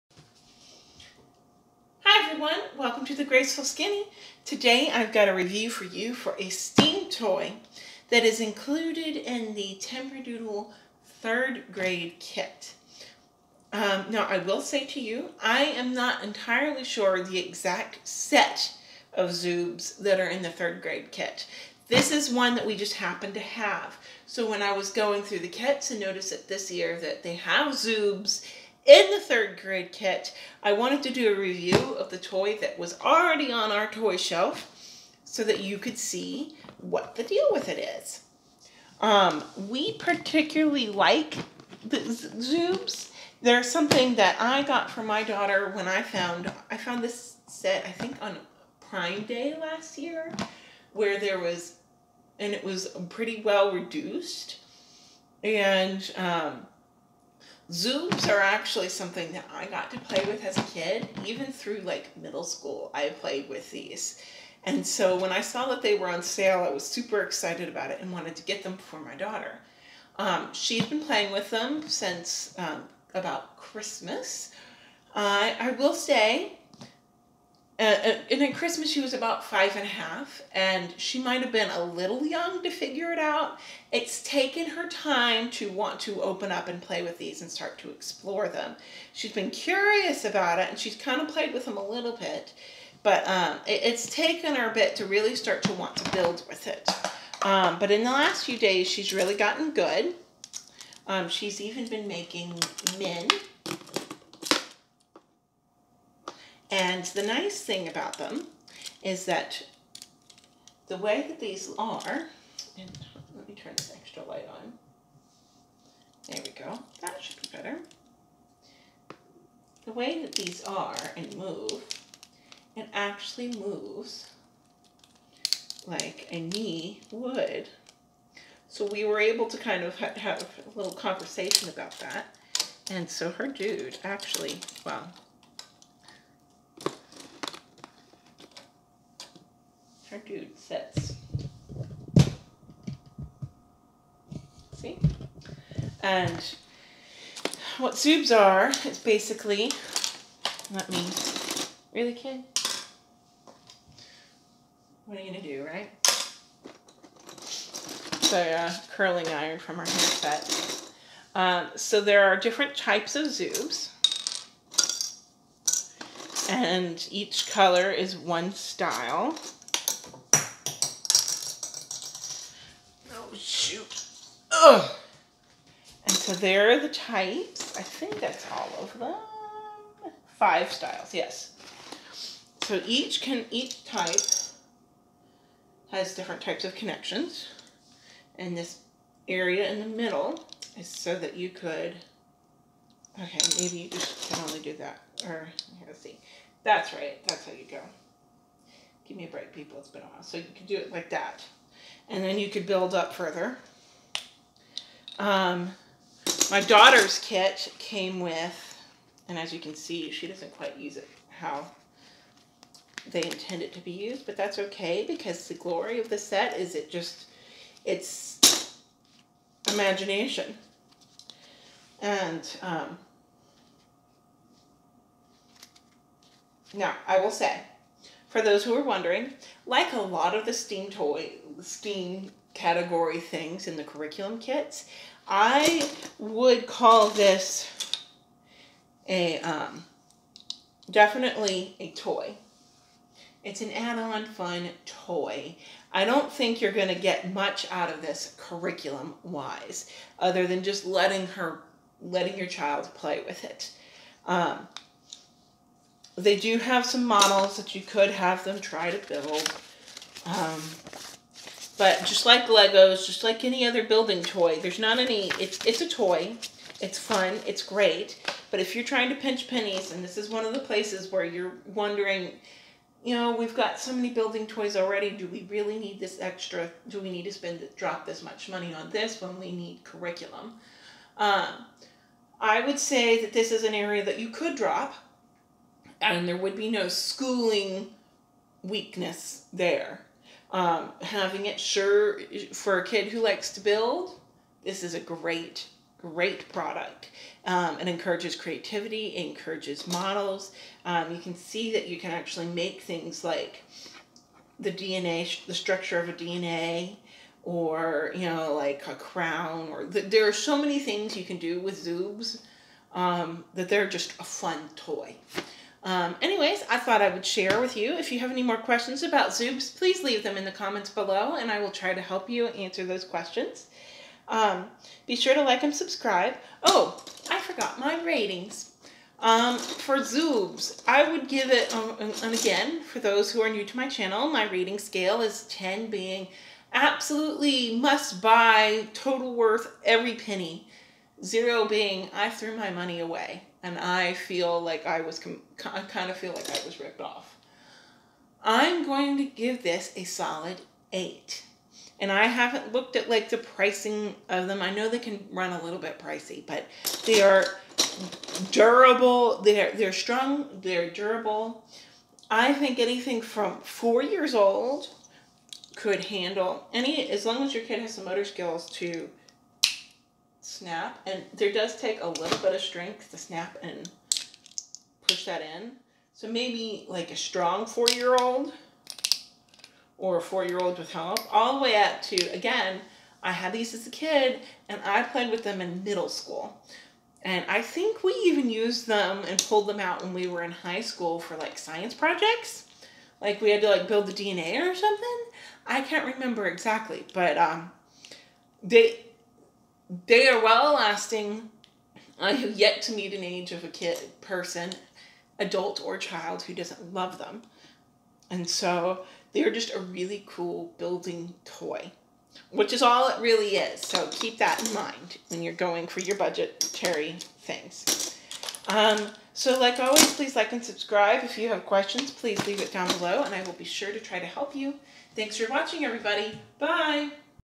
Zoob review
A  description and review of the Building and STEM toy, Zoobs. This toy is part of timberdoodle's 3rd-grade curriculum kit.